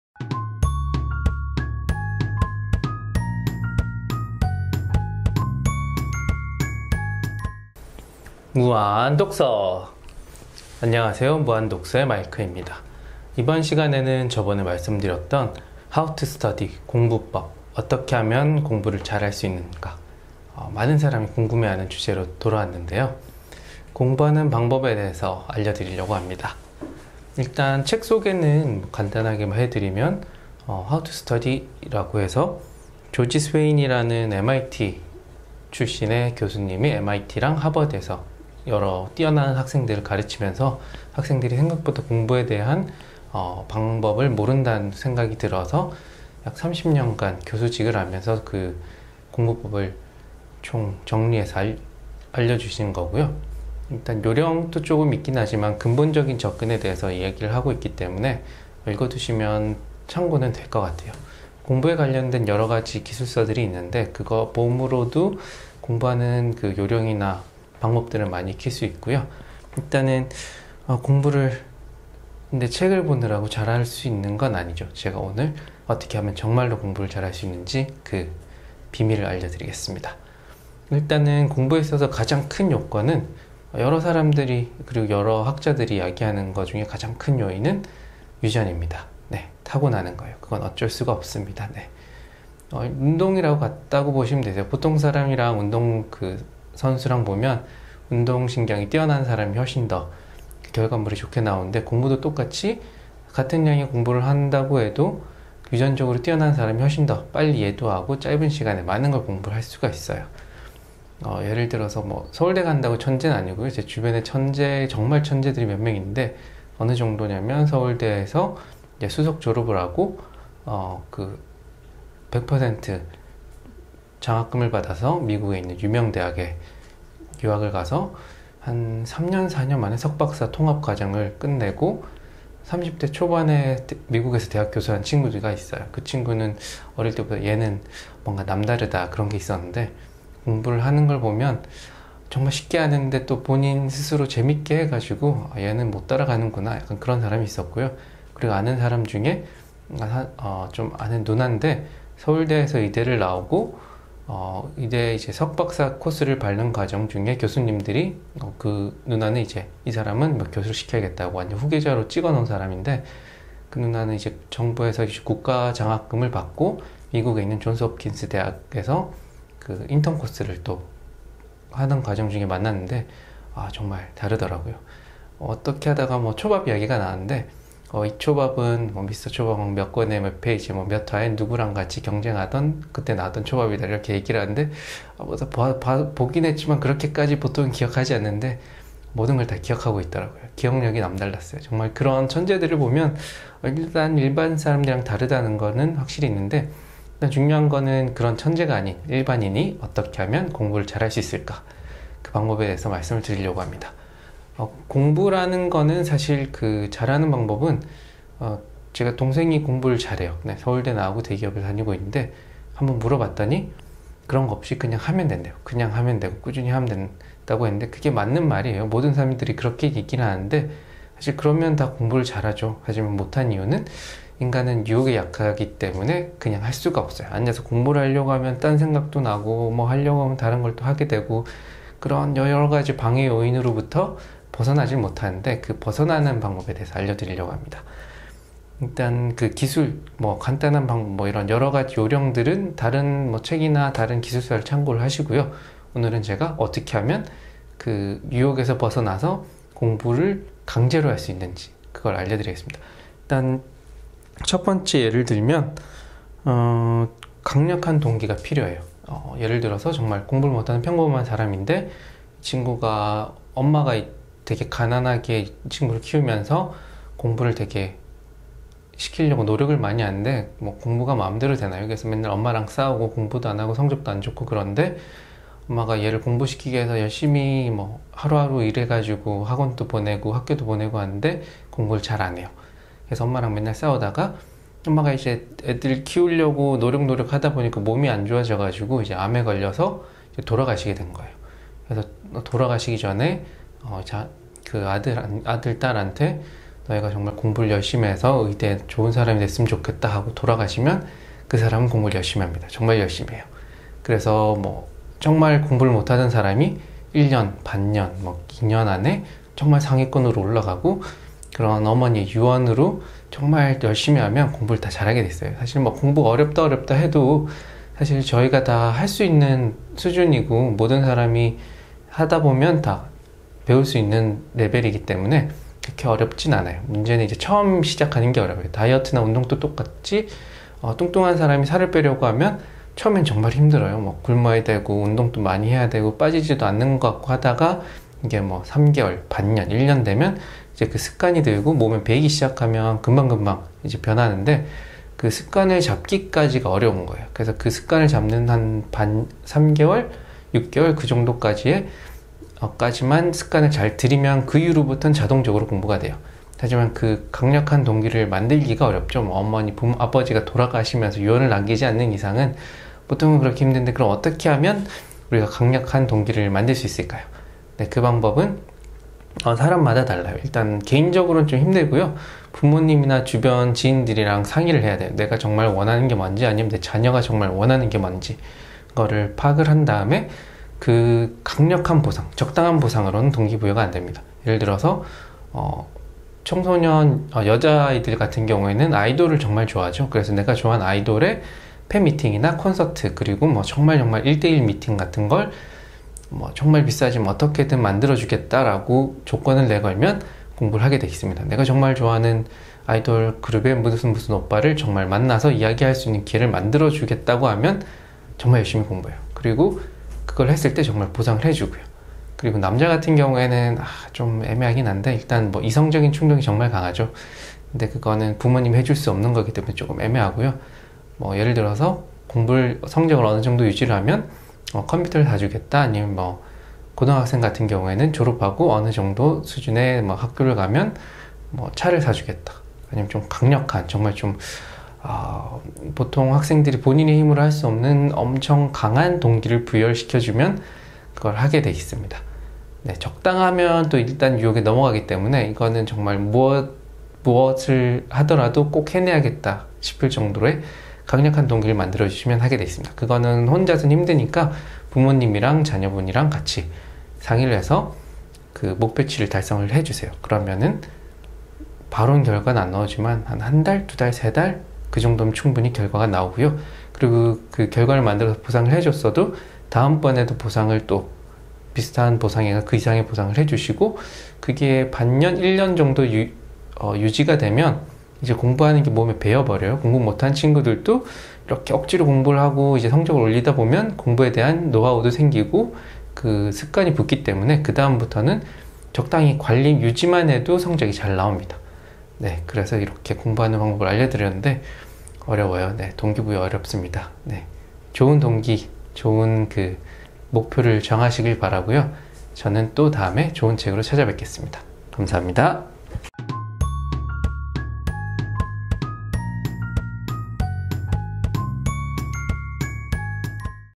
무한독서 책리뷰, How to study 공부책. 공부 잘 하는 방법. 공부 못하는 학생이 잘 할수 있는법. 학부모님들 필이 시청!!
How to study 공부책. 조지 스웨인 MIT 하버드 교수.
어떻게 하면 공부를 잘 할수 있을까요?
이런 저런 방법도 많고 이것저것 시도 해도 안되는 사람들을 위한 공부법을 알려드립니다